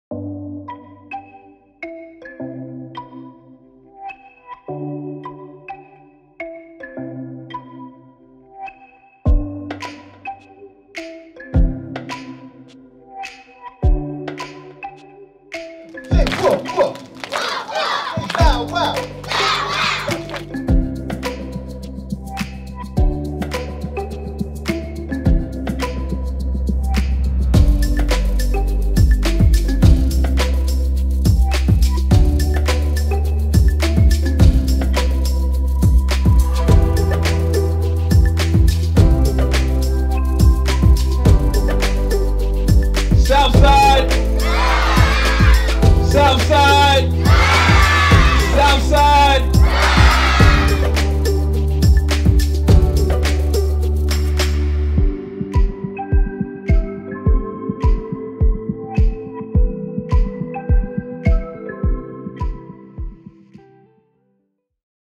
CalKids at Rosa Parks
On October 11, students of Rosa Parks K-8 School welcomed California Treasurer Fiona Ma, Assemblymember Stephanie Nguyen, CDE Chief Deputy Superintendent Mary Nicely, and the Mott Foundation to celebrate the first anniversary of the California Kids Investment and Development Savings Program (CalKIDS). The CalKids program is a financial investment in the future of California children designed to inspire them, especially those from underserved and disadvantaged communities, to dream big and pursue a college education or career training program after high school.